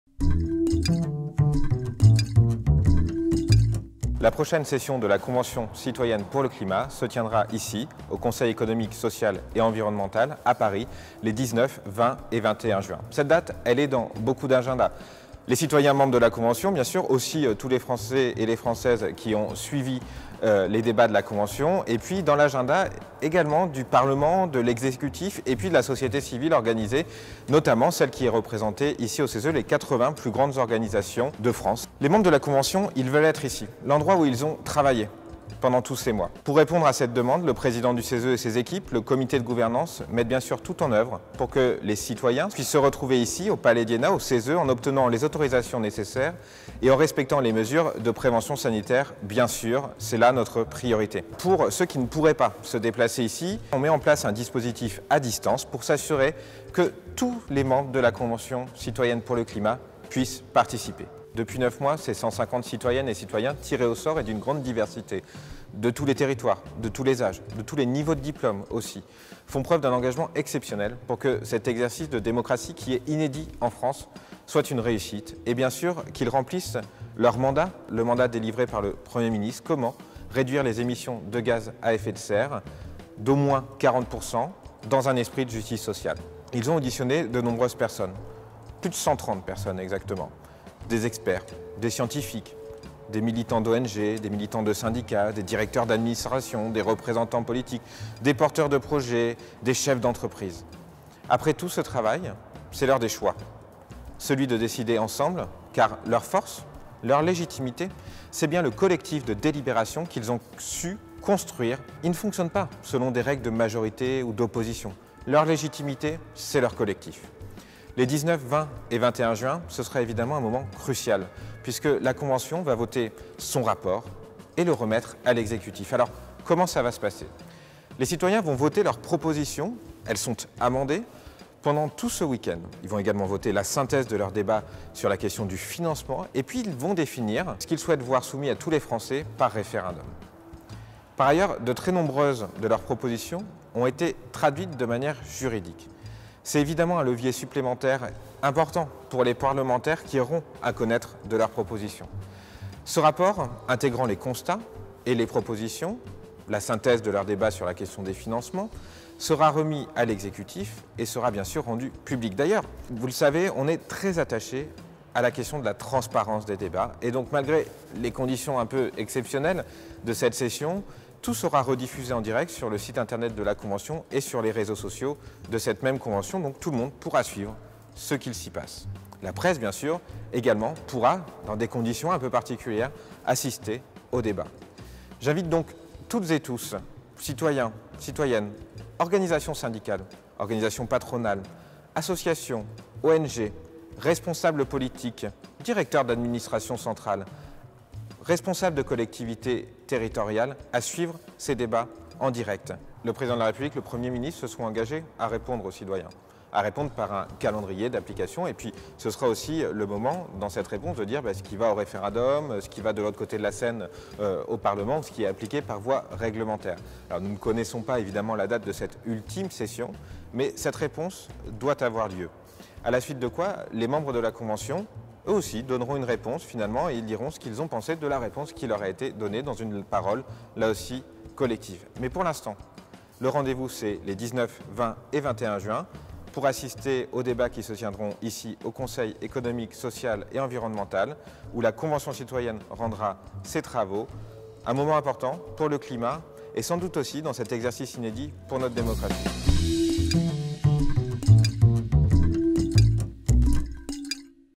Présentation de la session 7 Convention citoyenne pour le climat
Julien Blanchet, Rapporteur général de la Convention et vice-président du CESE rappelle que «Depuis 9 mois, ces 150 citoyennes et citoyens font preuve d'un engagement exceptionnel pour que cet exercice de démocratie, qui est inédit en France, soit une réussite».
Précisions sur la session 7 qui commence vendredi au CESE.